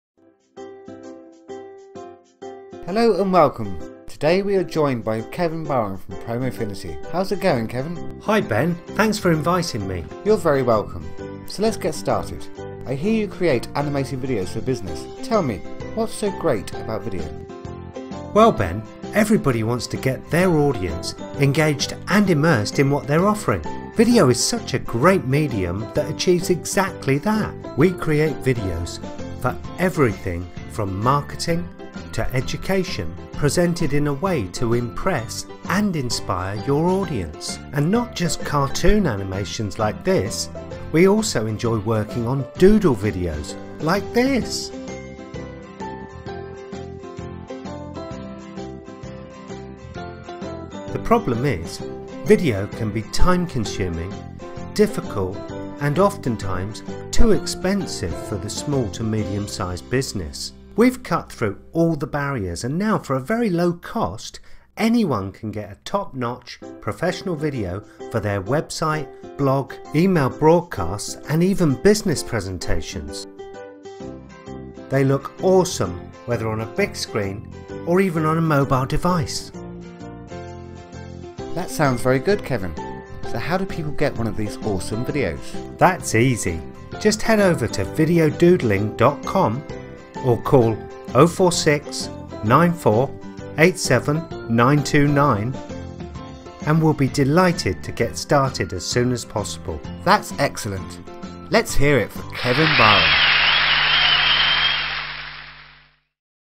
Promofinity Cartoon Video Animation
Hello & Welcome. (Ben Jones - Cutting Edge Media)

Today we are joined by Kevin Barham from Promofinity.  How's it going Kevin?

[Kevin] Hi Ben - Thanks for inviting me in.

[Presenter] You're very welcome.  So let's get started.  Tell me what's so GREAT about video.

[Kevin] Well Ben, everybody wants to get their audience, engaged and immersed in what they're offering -

Video is such a great medium that achieves exactly that.

We create videos for everything from marketing to education presented in a way to impress and inspire your audience.

...and not just cartoon animations, we also enjoy working on doodle videos.

The problem is that video can be time-consuming, difficult and often-times, too expensive for the small to medium size business.

We've cut through all the barriers and now, for a very low cost, anyone can get a top-notch, professional video for their website, blog, email broadcasts, and with output upto 1080p, business presentations.  They look awesome whether on a BIG screen or a mobile device.

[Presenter] That all sounds very good Kevin - So how do people get one of these awesome videos?

[Kevin] Just head over to VideoDoodling.com or call and we'll be delighted to get started as soon as possible.

[Presenter] That's excellent - Let's hear it for Kevin Barham...